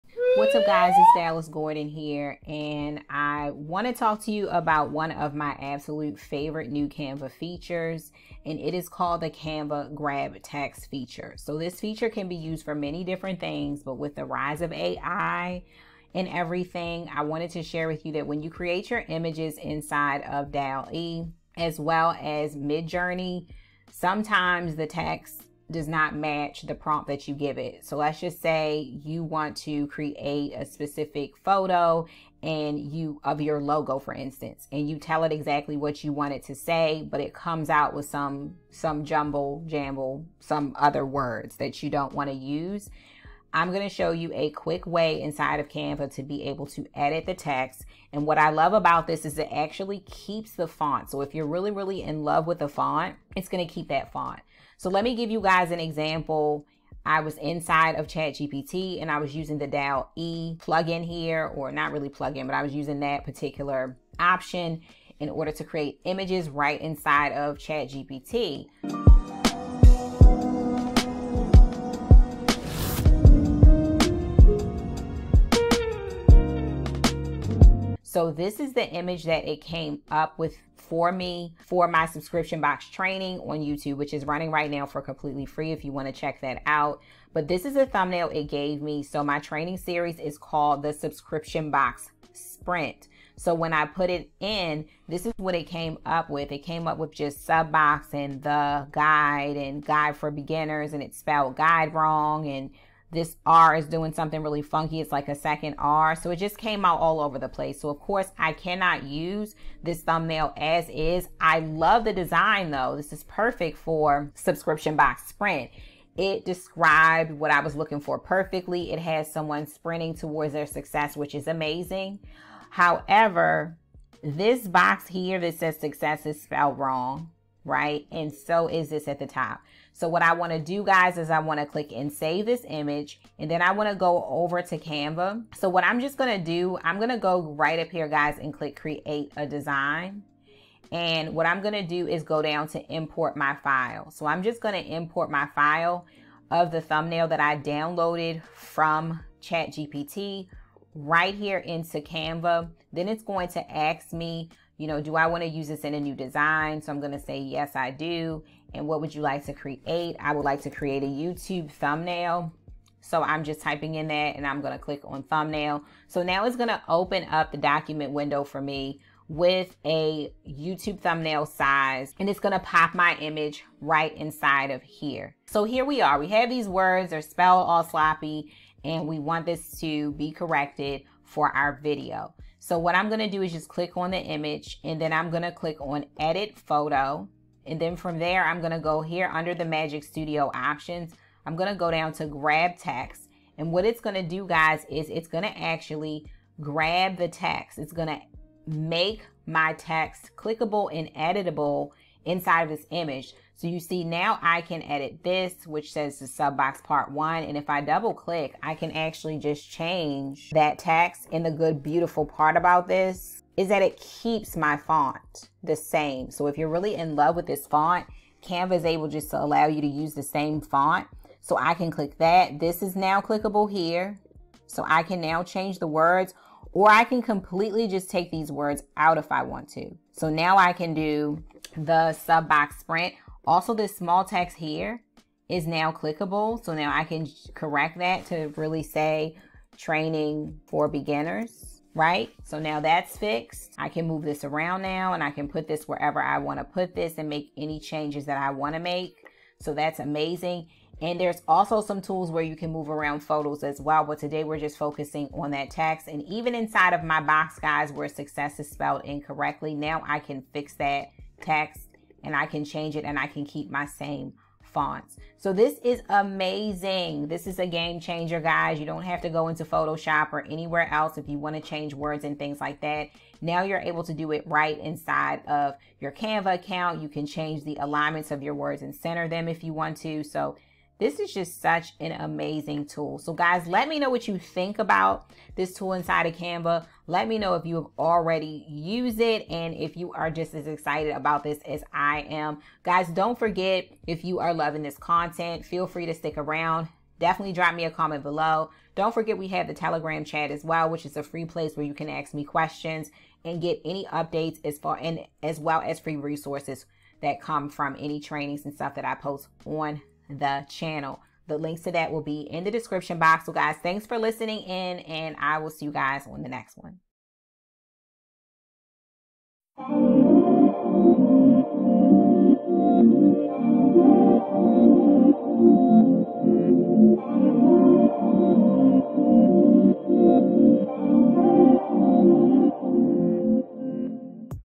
How to Use Canva Grab Text Feature
Hey Guys! I’m really excited about this new feature: Canva Grab Text

You can grab the text out of any photo and it also allows you to keep the remaining font and match it perfectly if needed.

This works perfect for Dall-E and Midjourney images as well as sometimes it’s hard to get the text right.